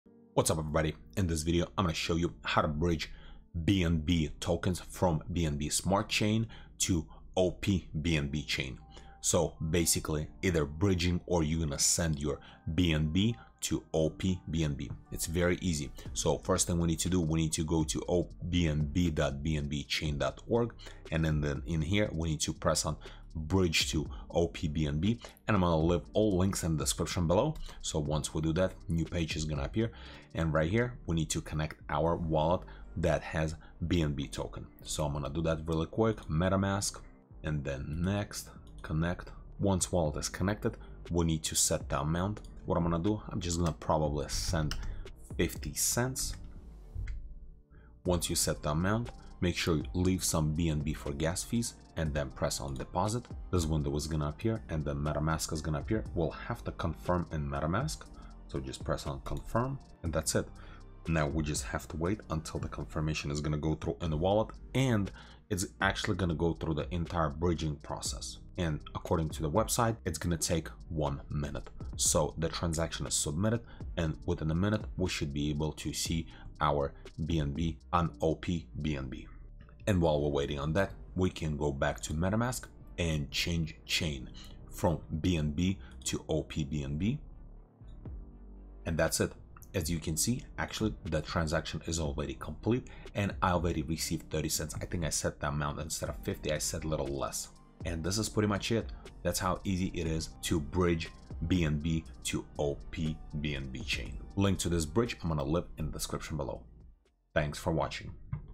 How to Bridge BNB to opBNB | Send BNB to opBNB | Step by Step Guide
In this video I will show you how to bridge BNB to opBNB, or send BNB to opBNB.

PERSONAL GEAR
4k Asus Monitor
Video Camera Tripod